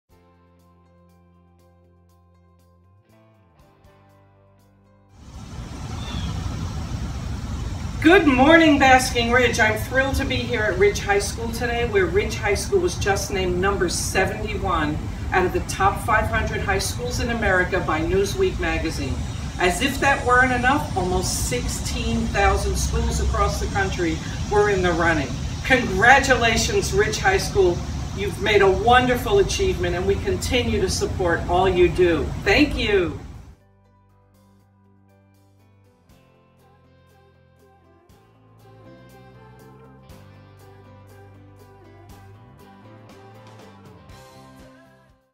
Congratulations, Ridge High School
Our own Basking Ridge's Ridge High School has been named number 71 out of the top 500 public high schools (out of more than 16,000 nationally) by Newsweek Magazine. Congratulations Ridge High School Students and Faculty!